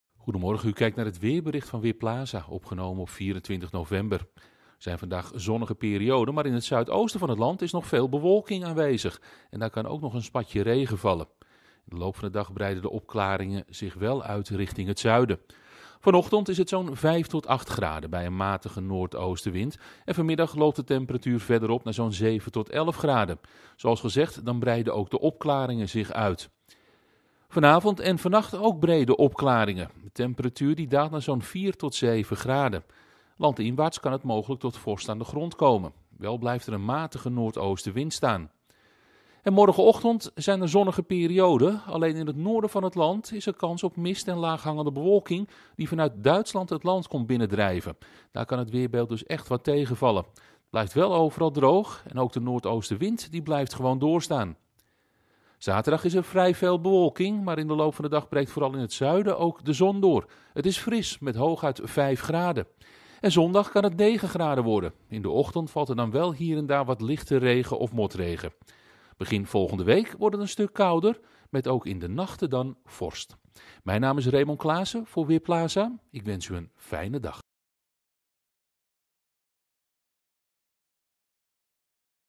Weerbericht donderdagochtend:Zonnige perioden
In het zuiden van het land is het bewolkt en vooral in Limburg valt soms nog wat regen. In het noorden schijnt de zon vandaag. In de loop van de dag breiden de opklaringen zich uit richting het zuiden. Vanmiddag wordt het 7 tot 11 graden bij een matige tot vrij krachtige noordoostenwind. Morgen wordt het maximaal 7 graden. Er is veel zon en de wind neemt iets af. Zaterdag wordt de wind noordelijk en drijft er meer bewolking binnen. Het blijft meest droog en het wordt dan 5 graden.